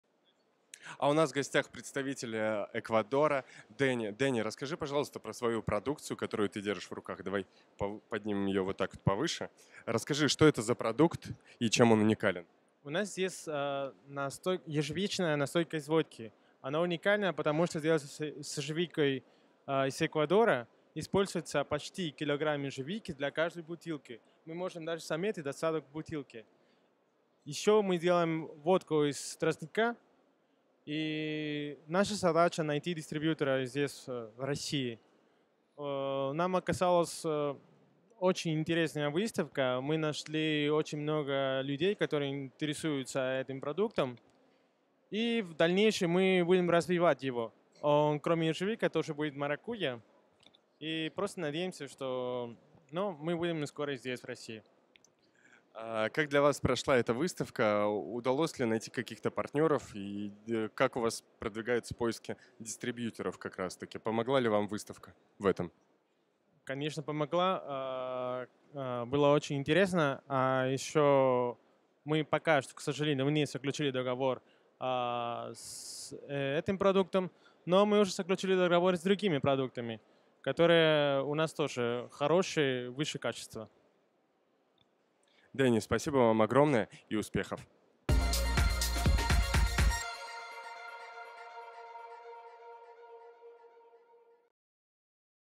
Danny Tituana, PRO ECUADOR
Danny Tituana, PRO ECUADOR, рассказал о продукции, ее уникальности, поделился впечатлениями о выставке.